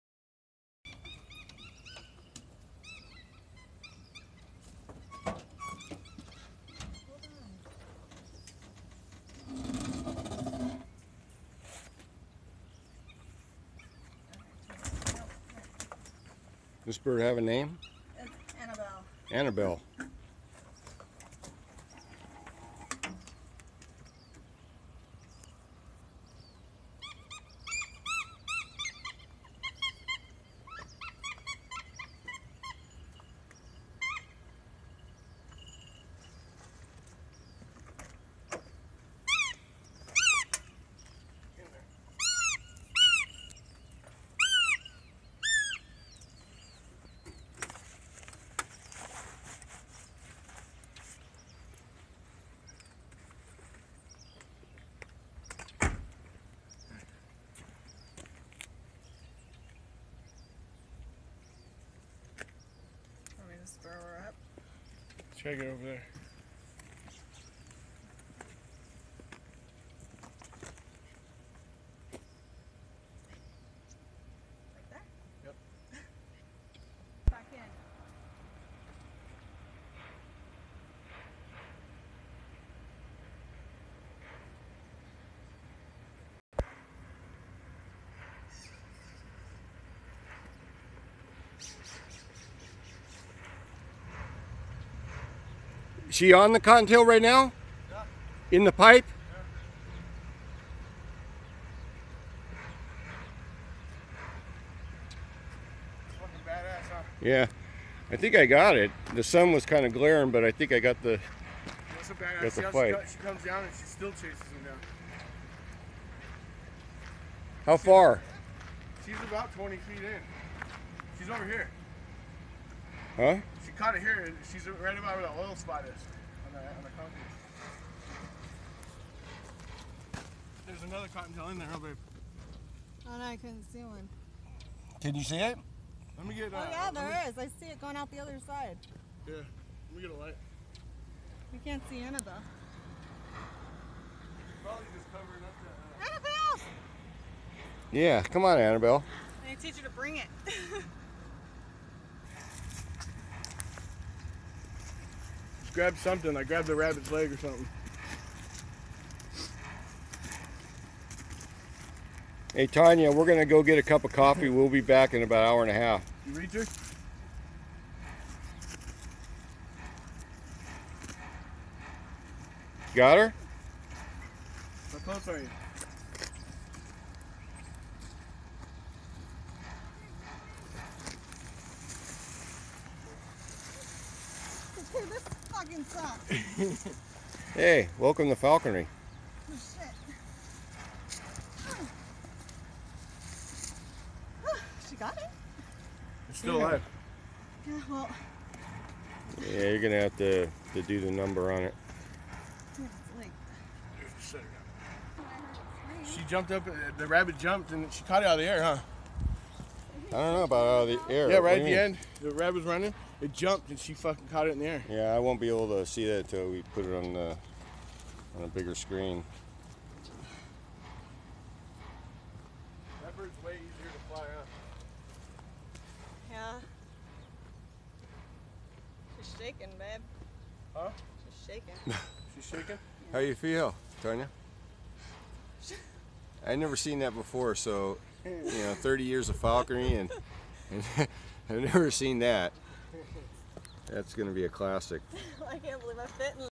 Tanya and her Red Tail 'Annabelle' hunting
Annabelle catches first Cottontail Rabbit. Pleasant Grove, Ca. 9/27/15